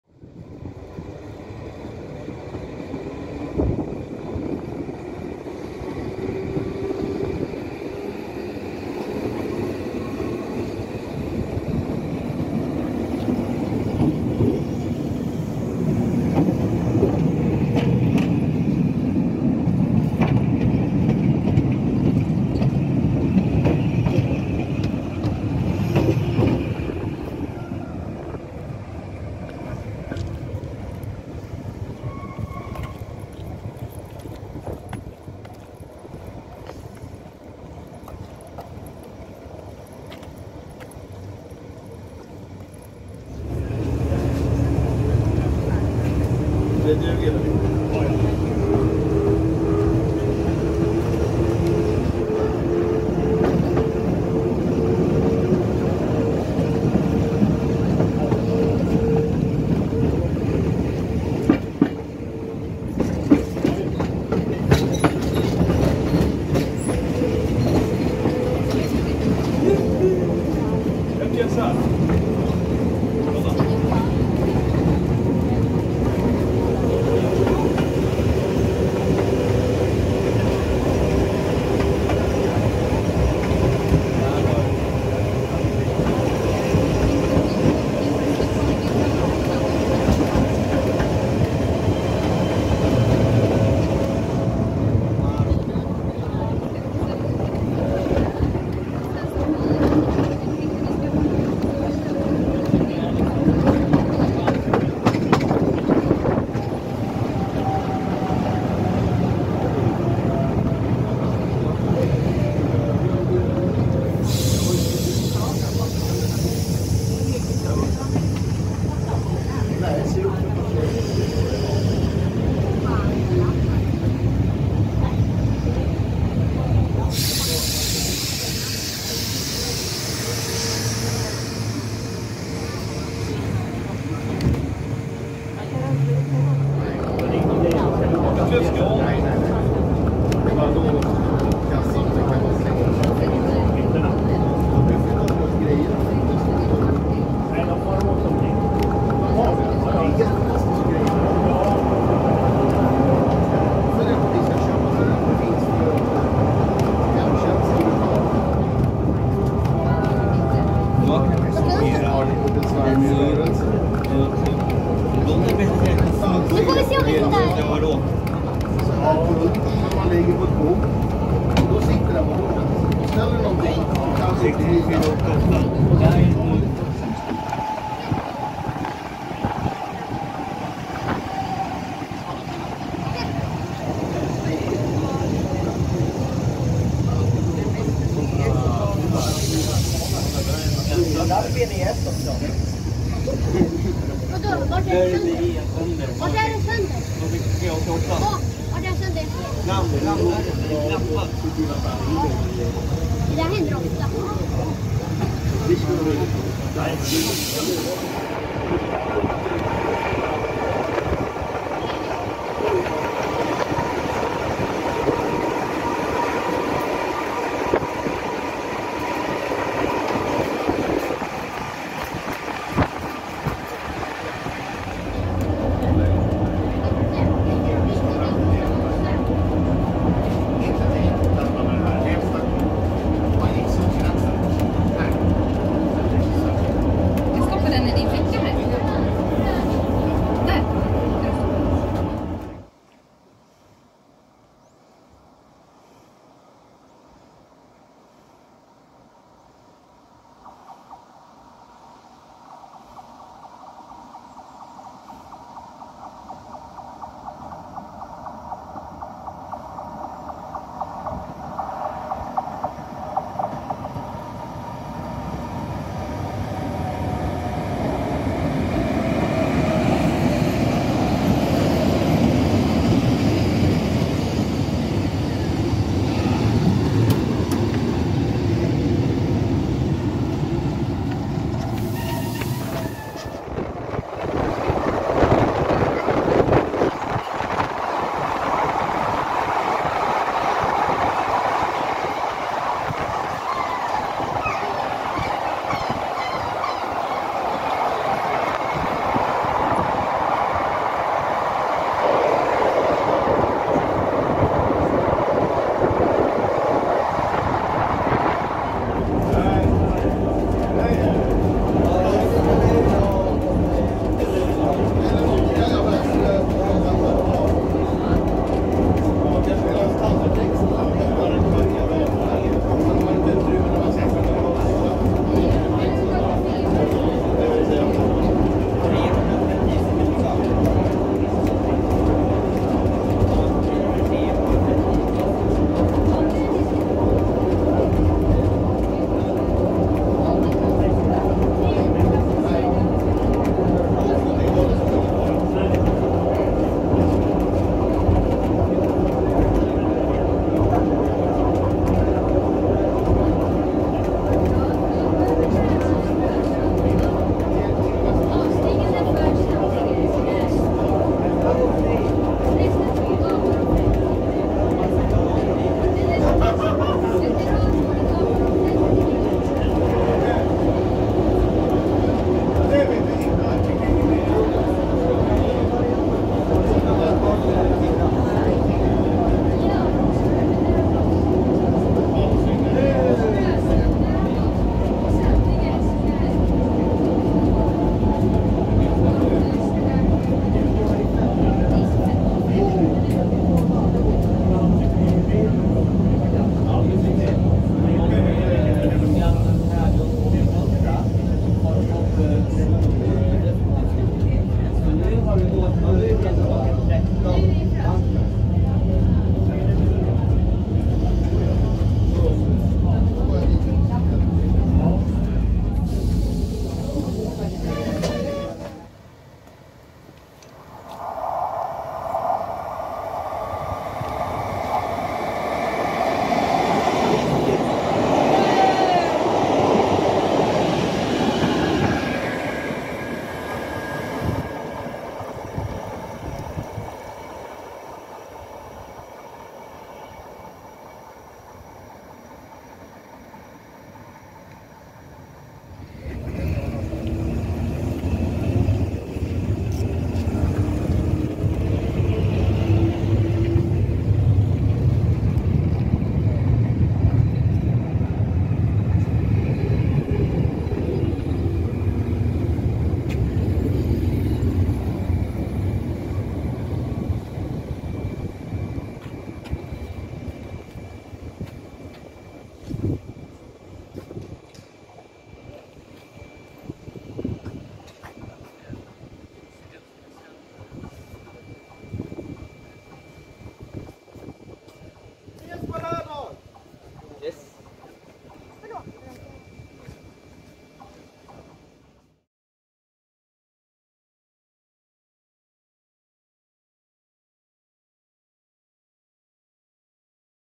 Veterantåg på Roslagsbanan 8/9/2024
Den 8:e september 2024 så gick roslagsbanans veterantåg som ett extratåg från Stockholms östra till Frösunda sedan tillbaka och togs ur trafik, tåget kördes alltså inte till Österskär.

Inspelat: 8:e september 2024

Filmat med: @mr.c6-1312 @mr_cxspotter1384 @busestrainsinswedenandmore @danielkollektivtrafik @SL-kanalen. @Swebus-Trainspotter @Tag308